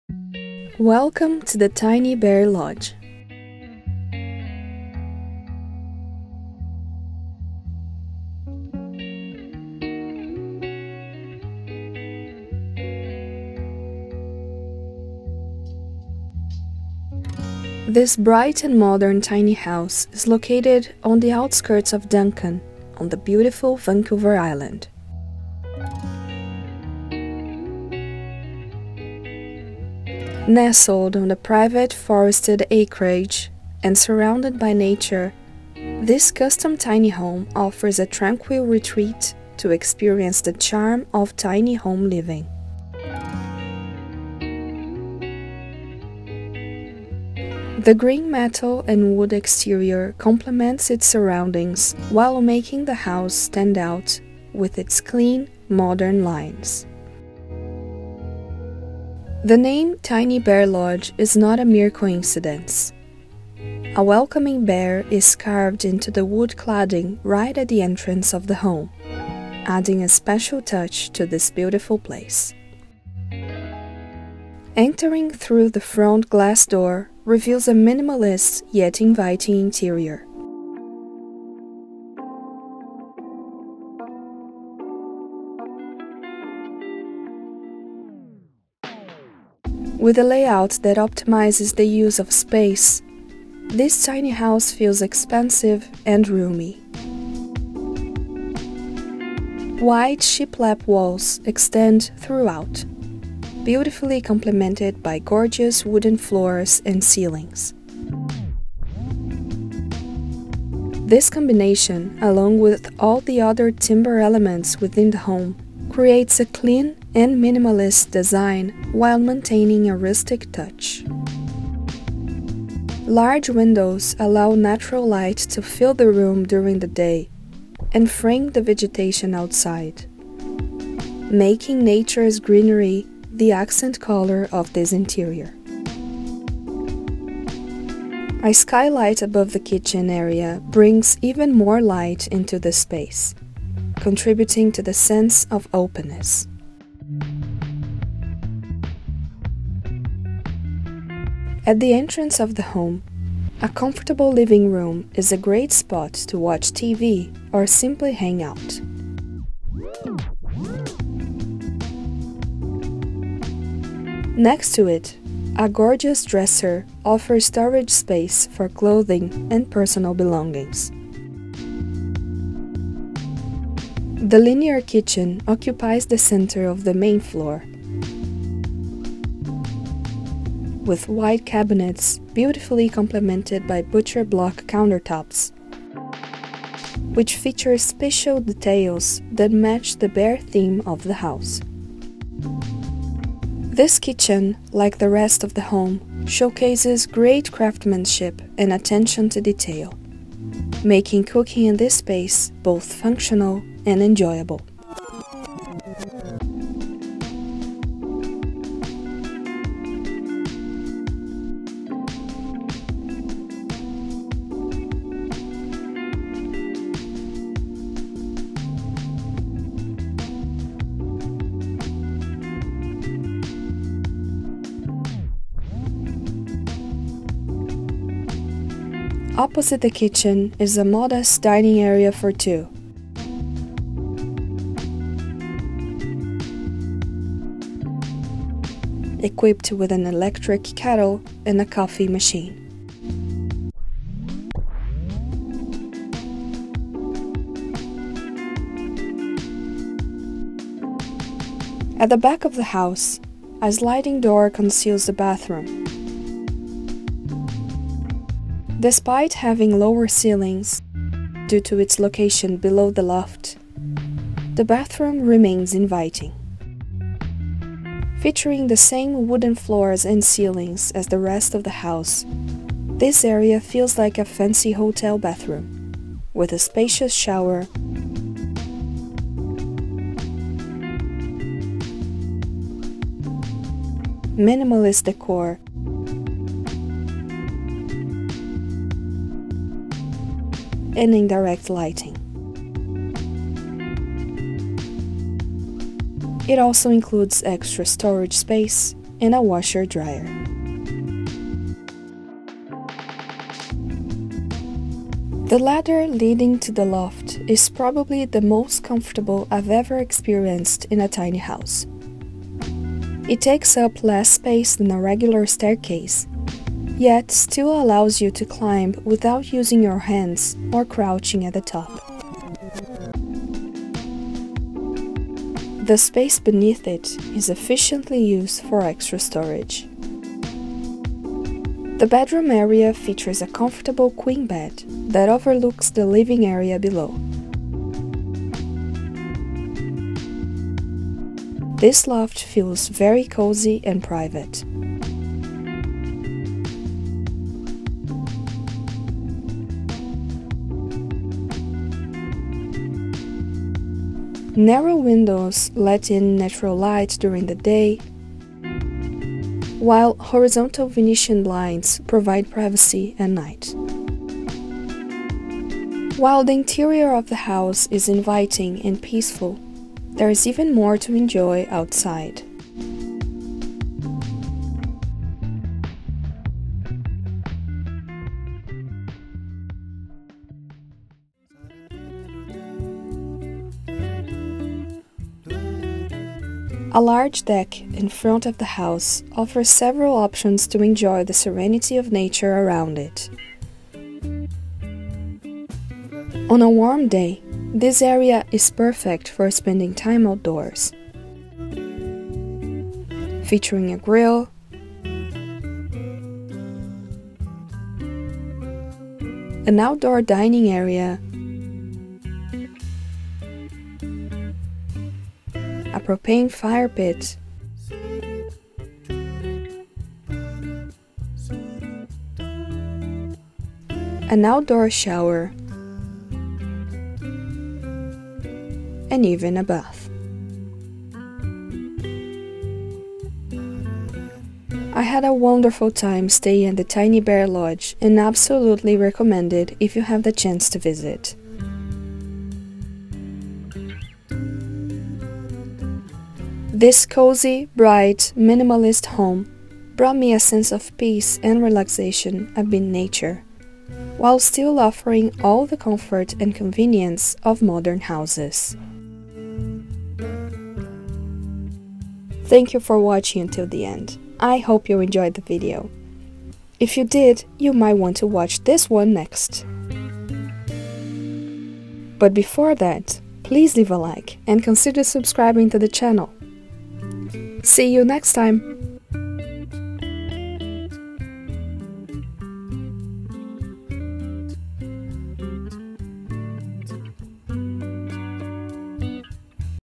Inside a Bright and Modern Tiny House with Outstanding Craftsmanship
This tiny house is absolutely stunning! It has an efficient layout and looks amazing, with modern and bright decor.
Enjoy the tour!